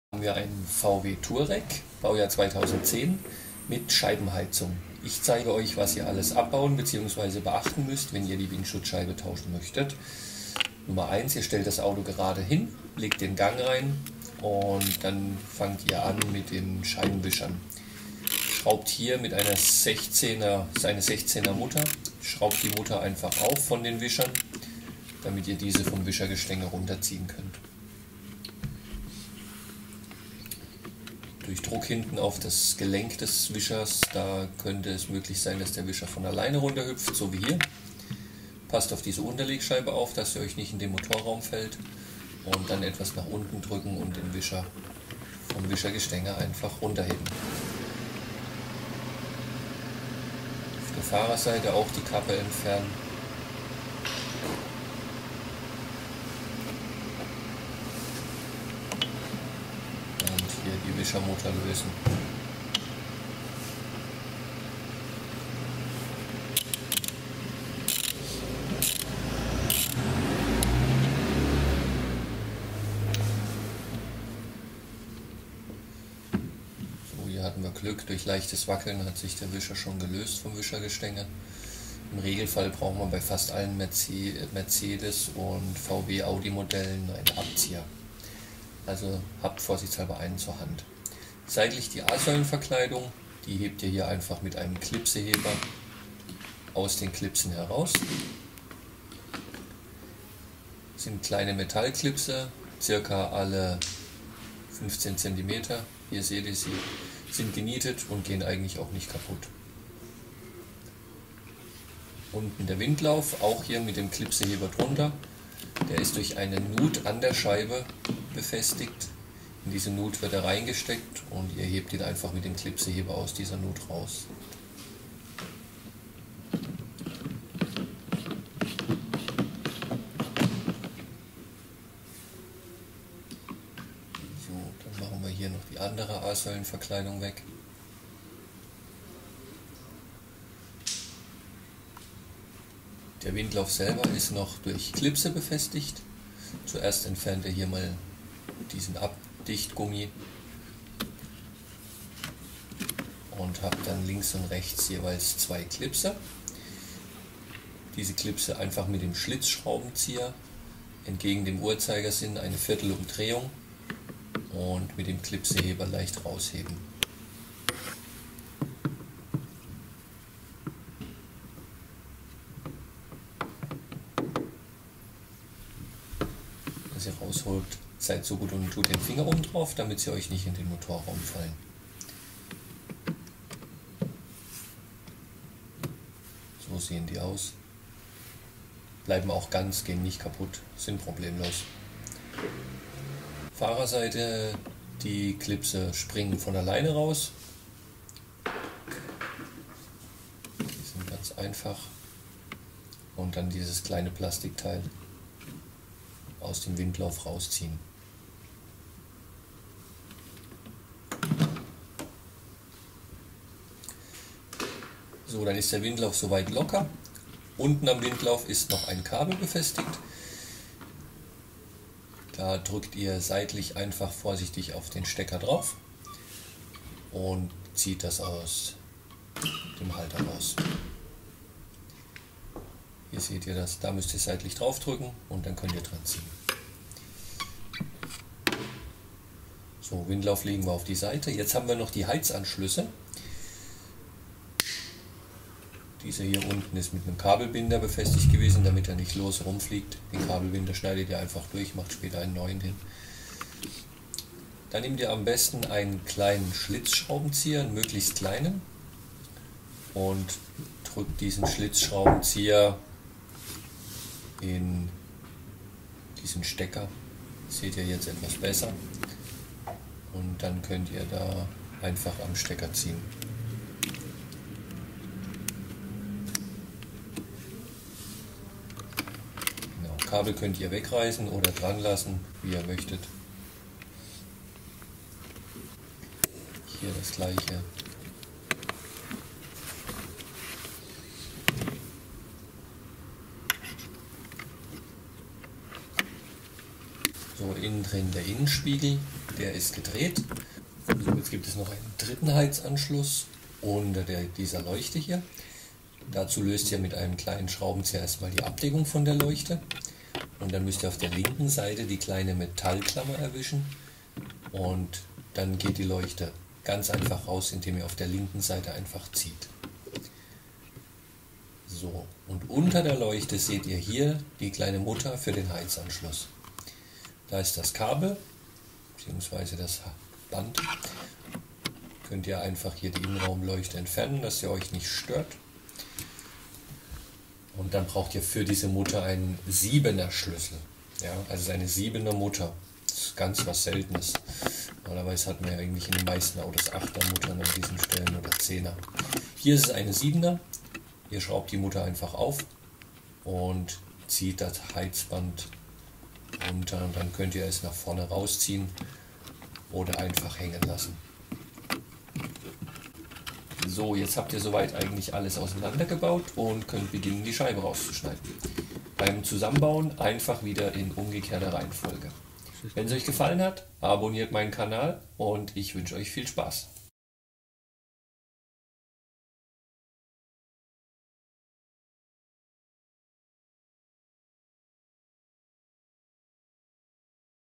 VW Touareg Scheibenheizung Bj.2010 Frontscheibe tauschen, was müsst Ihr abbauen, ausbauen, beachten
Der Ausbau einer Frontscheibe und die wegzubauenden Teile bis die Scheibe für den Ausbau freiliegt an einem VW Touareg TDI 3.0 Bj. 2010 mit Scheibenheizung Windschutzscheibe bzw. Frontscheibe ausbauen, Windlauf, Scheibenwischer, Klipse, A-Säulen Verkleidung und vieles mehr muss man ausbauen, bevor die Scheibe problemlos rausgeschnitten werden kann. Was Ihr beachten solltet! Tipps und Tricks für den Scheibentausch für Selbstversuche, Hobbyhandwerker und Profis. Ich hoffe es hilft. Für Fragen gerne kommentieren.

Euer Schwabo Autoglas Team Karlsruhe